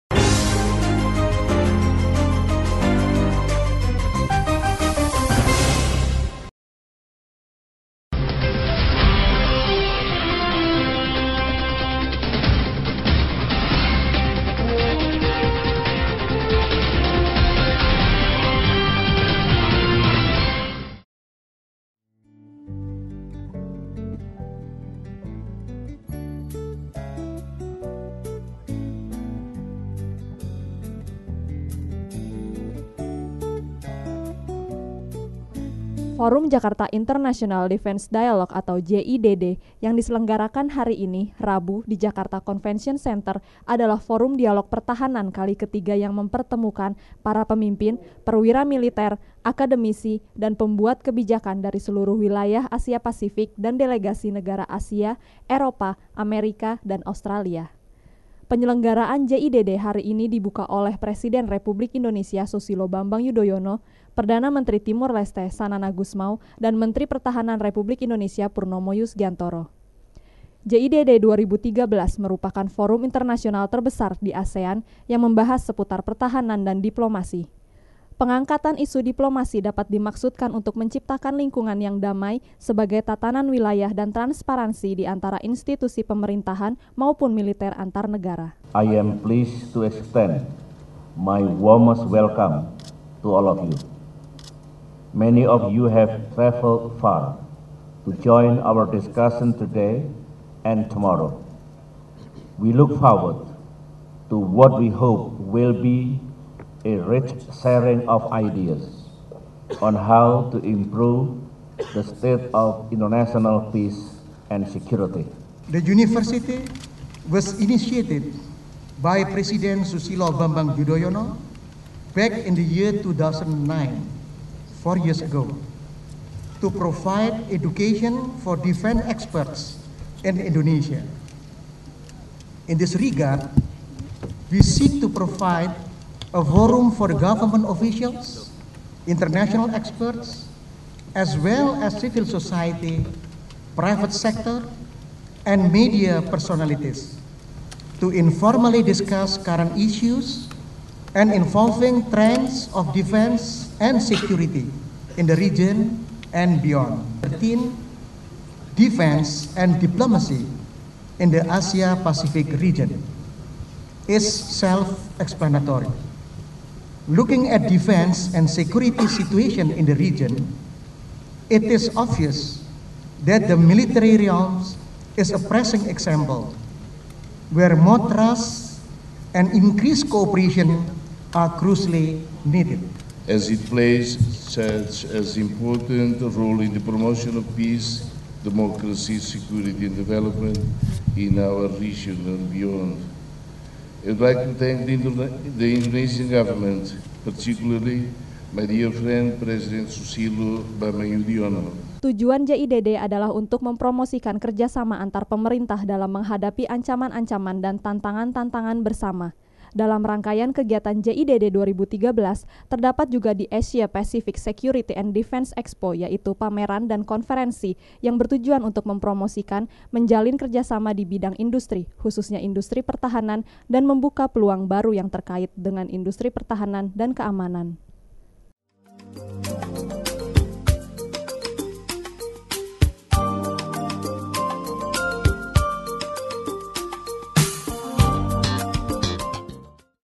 Jakarta International Defence Dialogue (JIDD) 2013
Forum Jakarta International Defence Dialogue (JIDD) yang diselenggarakan hari ini, Rabu di Jakarta Convention Centre adalah Forum Dialog Pertahanan kali ketiga yang mempertemukan para pemimpin, perwira militer, akademisi dan pembuat kebijakan dari seluruh wilayah Asia Pasifik dan delegasi Negara Asia, Eropa, Amerika dan  Australia.
Penyelenggaraan JIDD hari ini dibuka oleh Presiden Republik Indonesia Susilo Bambang Yudhoyono, Perdana Menteri Timor Leste Xanana Gusmao, dan Menteri Pertahanan Republik Indonesia  Purnomo Yusgiantoro. JIDD 2013 merupakan forum internasional terbesar di ASEAN yang membahas seputar pertahanan dan diplomasi. Pengangkatan isu diplomasi dapat dimaksudkan untuk menciptakan lingkungan yang damai sebagai tatanan wilayah dan transparansi diantara institusi pemerintahan maupun militer antar negara.
Tujuan JIDD adalah untuk mempromosikan kerja sama antar pemerintah dalam menghadapi ancaman-ancaman dan tantangan-tantangan bersama. Dalam rangkaian kegiatan JIDD 2013, terdapat juga the Asia-Pacific Security & Defence Expo yaitu pameran dan konferensi yang bertujuan untuk mempromosikan, menjalin kerjasama di bidang industri khususnya industri pertahanan dan membuka peluang baru yang terkait dengan industri pertahanan dan keamanan.